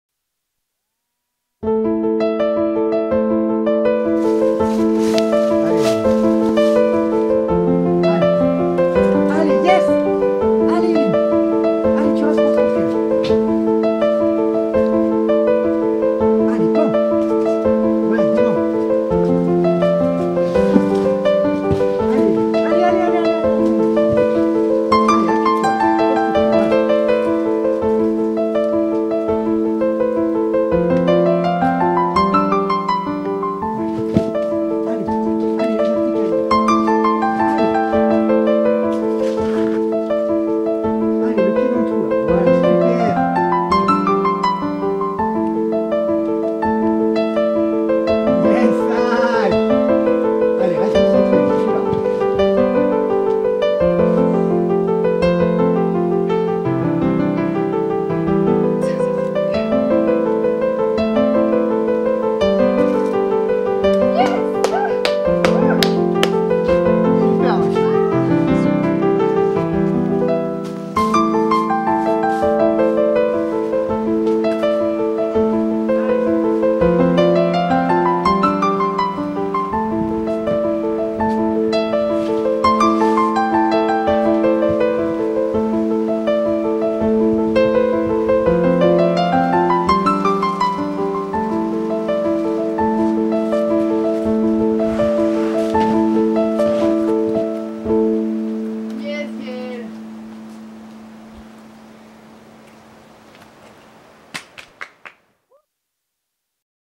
Annot 2017
Petite scéance avent Annot a Bloc.
Président Evil B8 (7A) pour Eline
Si tu dérappes ... B10 (7B) pour Gaël